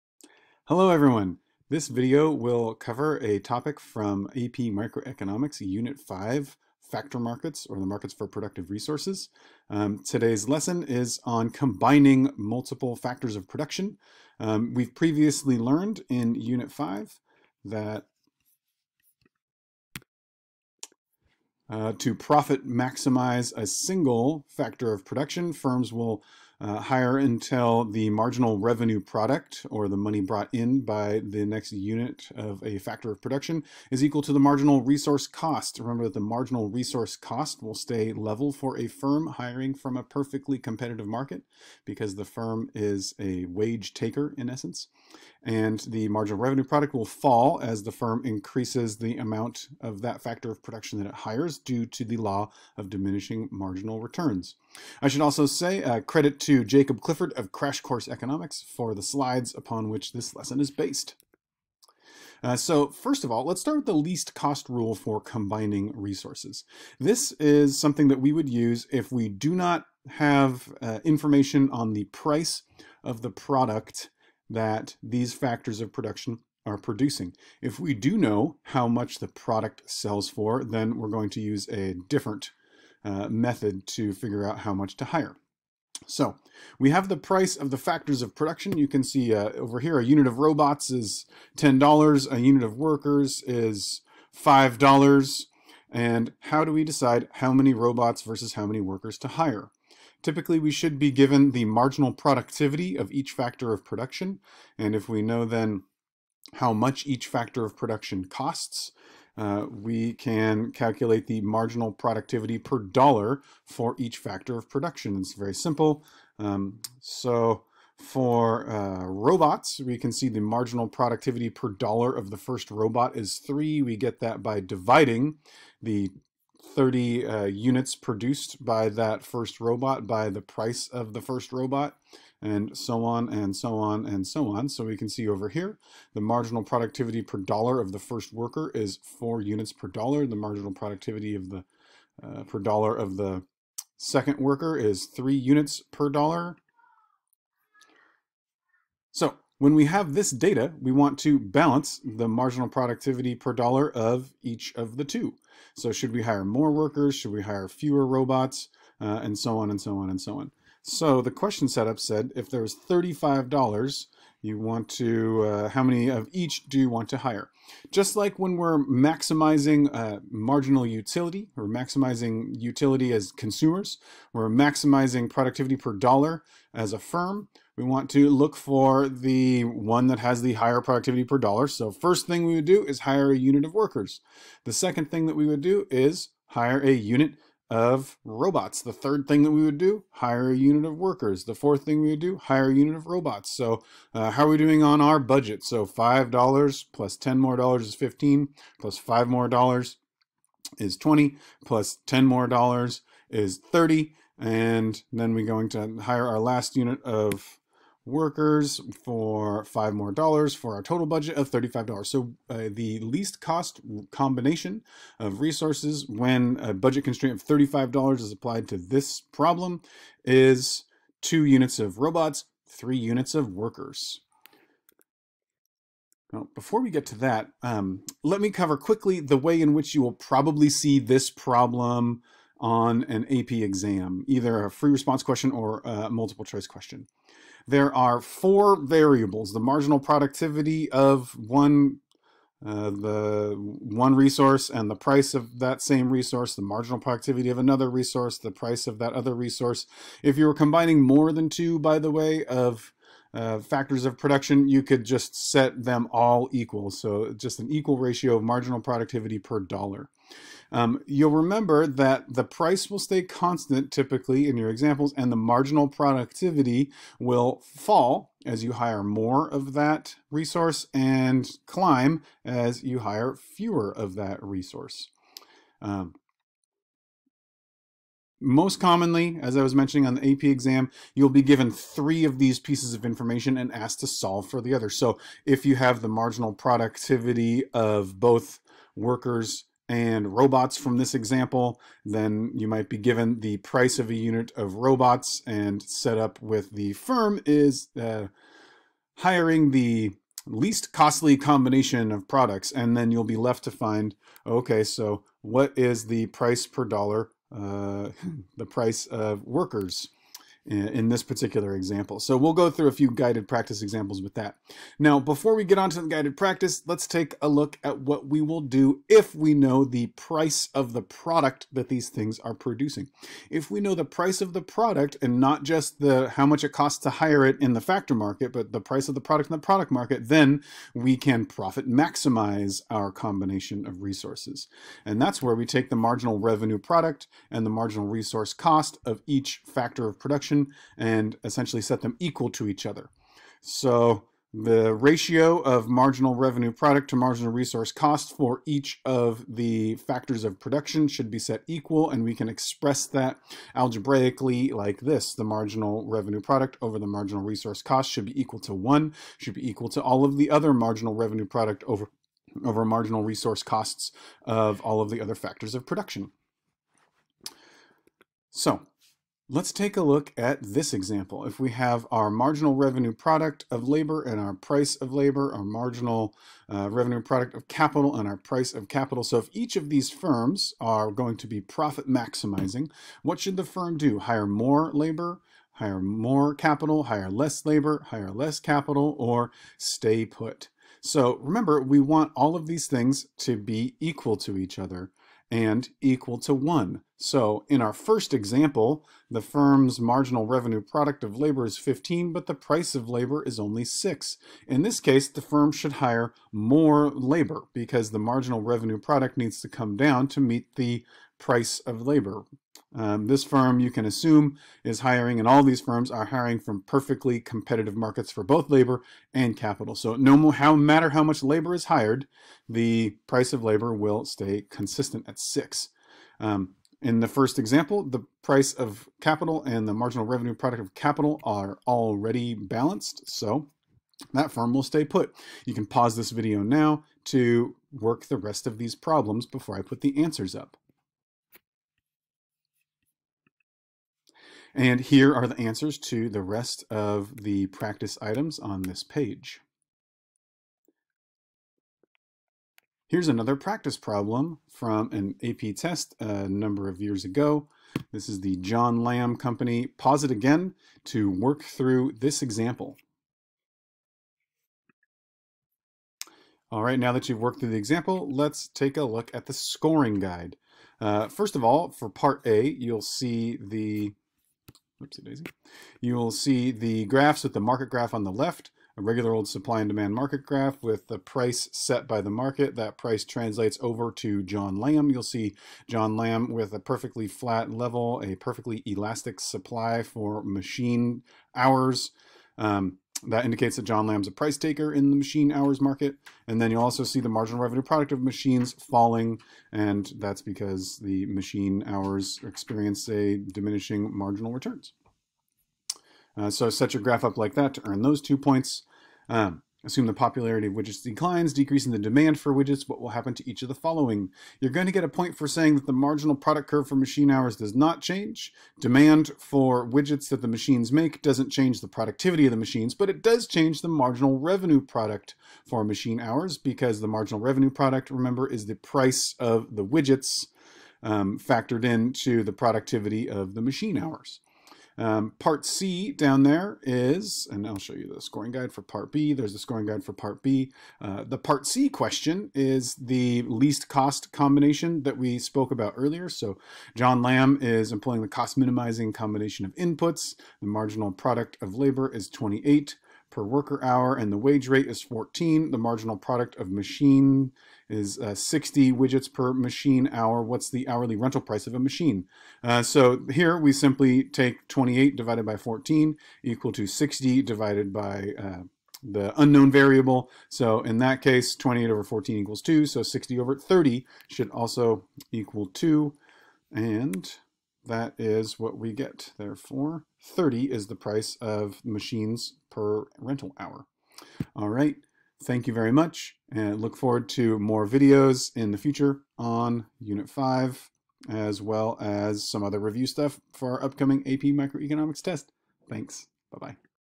AP Microeconomics Unit 5 - Combining Resources
A quick overview of both the Least-Cost method and the Profit Maximizing method for combining productive resources, followed by guided practice questions.

Thanks to Jacob Clifford for the slides on which this presentation is based.